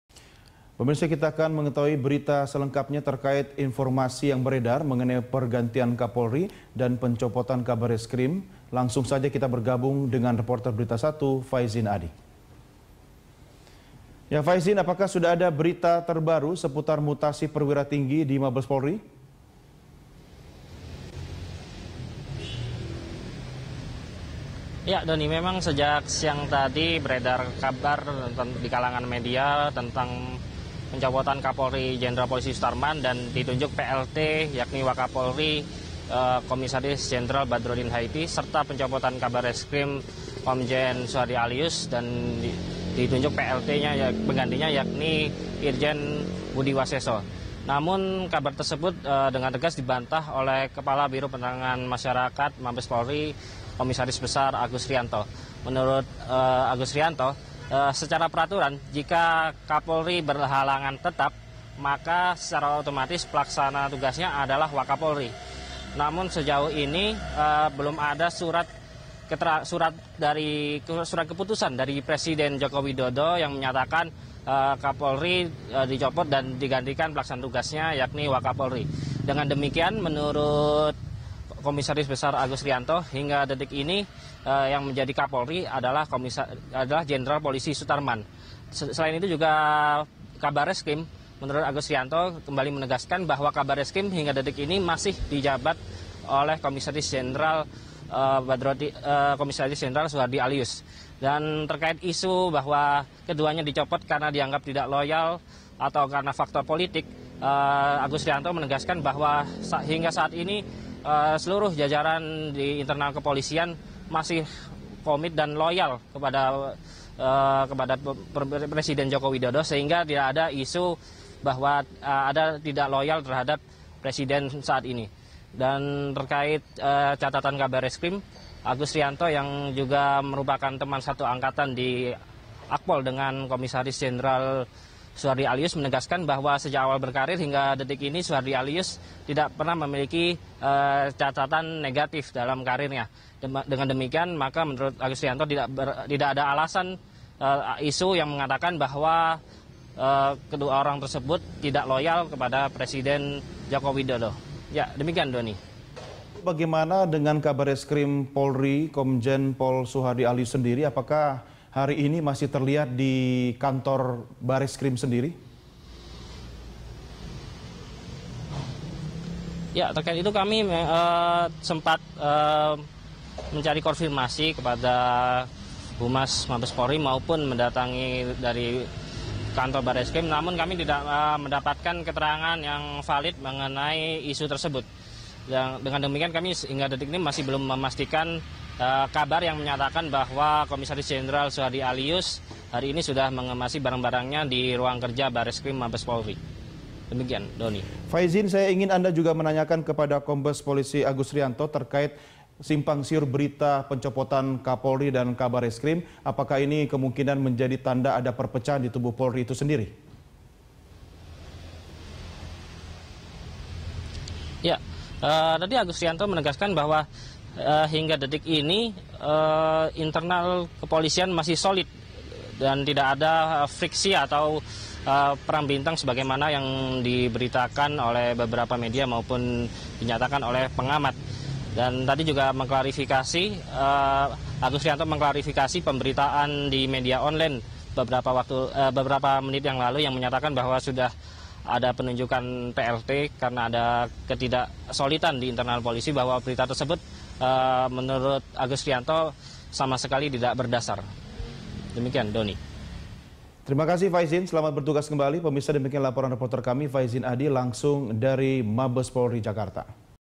Mabes Polri: Kapolri Masih Dijabat Jenderal Sutarman
Kepala Biro Penerangan Masyarakat Mabes Polri Kombes Pol Agus Rianto menegaskan belum menerima surat resmi mutasi perwira tinggi. Agus Rianto mengatakan Kapolri Jenderal Sutarman dan Kabareskrim Komjen Suhardi Alius masih loyal terhadap Presiden Jokowi.

Facebook.com/BeritaSatuTV
Youtube.com/BeritaSatu